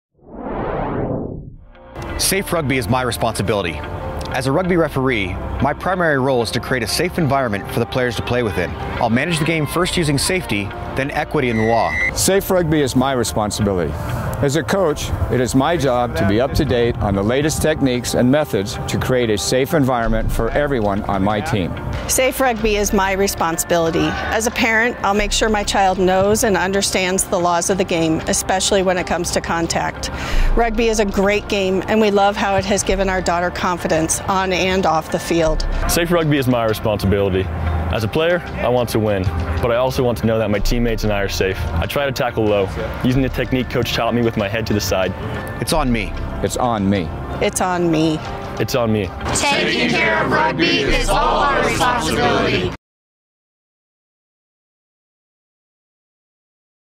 USA Rugby: Digital Safety Series
In a continued effort to promote player safety, USA Rugby will be releasing a series of videos aimed at educating all persons involved from the grassroots to national level.